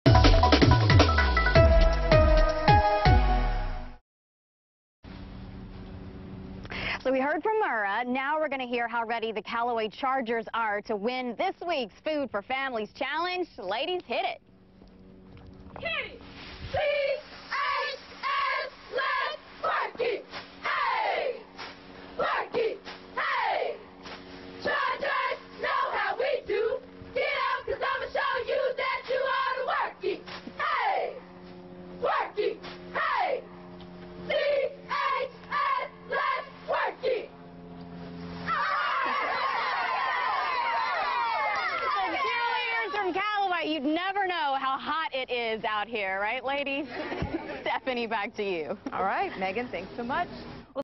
FFF: Callaway High School cheerleaders
Callaway takes on Murrah in the Food for Families challenge.